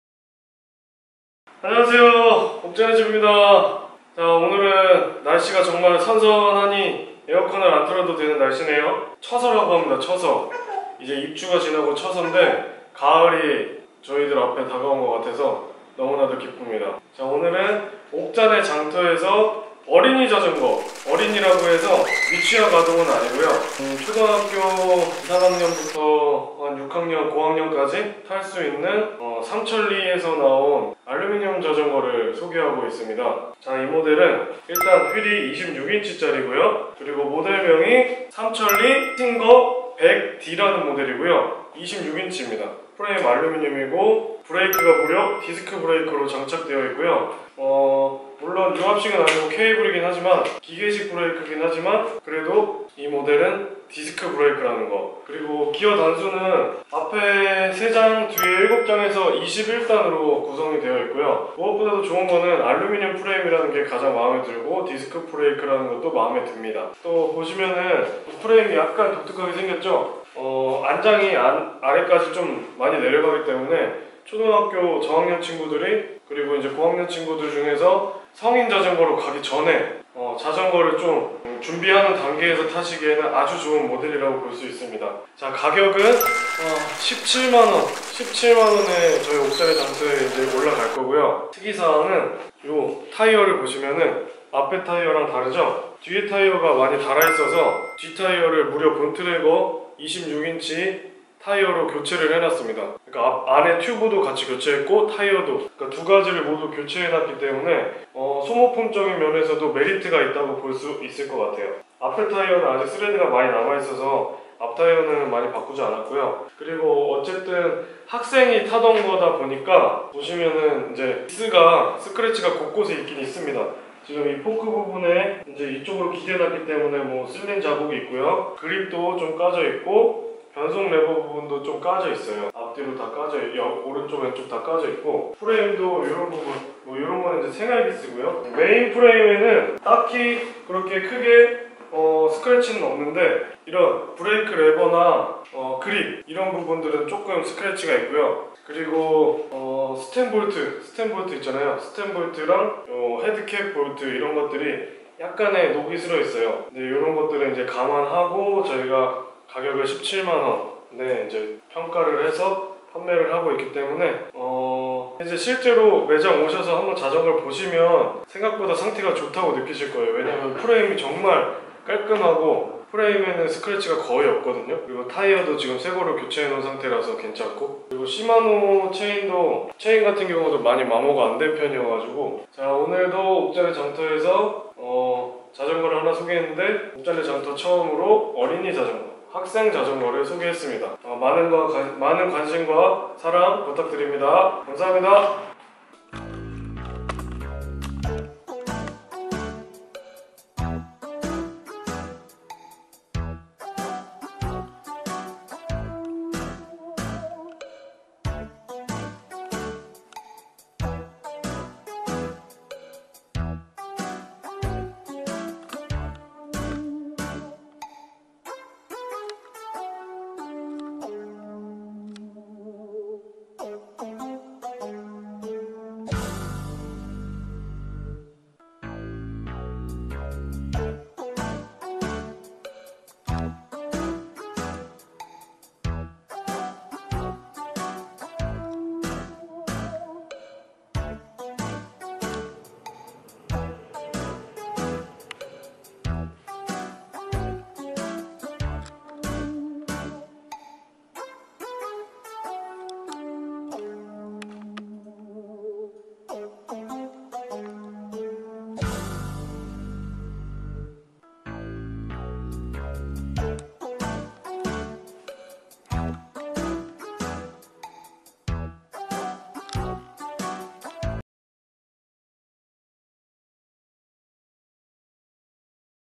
[판매완료][옥자네장터] 삼천리 26 스팅거 100D 17만원
[판매완료]
안녕하세요.
옥자네집:옥길자전거 입니다.

[옥자네장터] 5탄!!

삼천리 26 스팅거 100D 모델을 소개합니다.

시마노 구동계
알루미늄 프레임
타이어 상태 A+ (뒷타이어 새제품 본트래거로 교체)
21단 기어
큰 스크래치 없습니다.
초등학생 저학년부터 고학년까지 가능합니다.